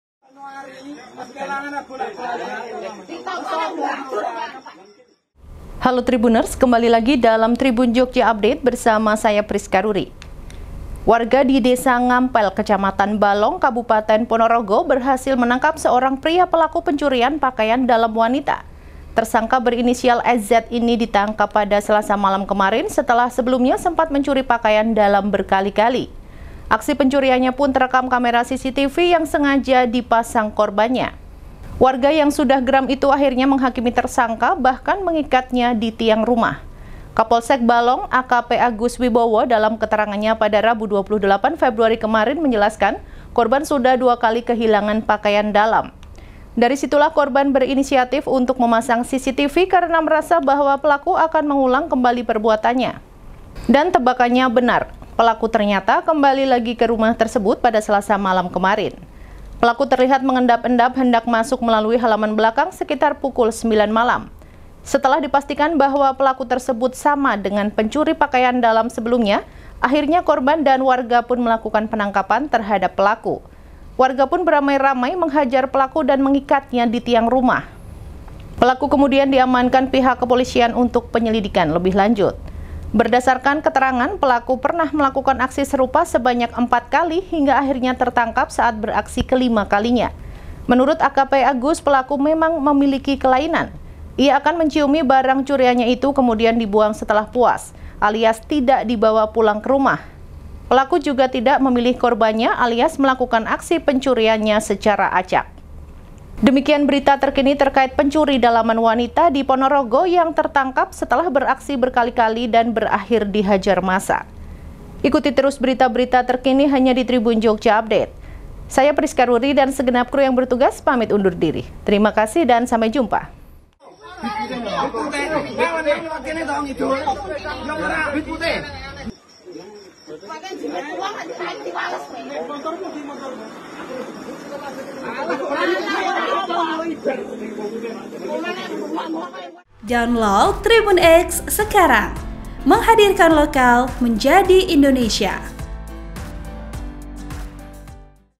Pencuri Daleman Tertangkap Setelah Beraksi Berkali kali, Akhirnya Dihajar Massa
Warga di Desa Ngampel, Kecamatan Balong, Kabupaten Ponorogo berhasil menangkap seorang pria pelaku pencurian pakaian dalam wanita.

Tersangka berinisial SZ ini ditangkap pada Selasa malam kemarin setelah sebelumnya sempat mencuri pakaian dalam berkali-kali.

Aksi pencuriannya pun terekam kamera CCTV yang sengaja dipasang korbannya.

Editor Video : Veri Vesiano
Narator : Prisca Ruri
Editor Naskah : Mona Kriesdinar
Produser : Ribut Raharjo
(vsn)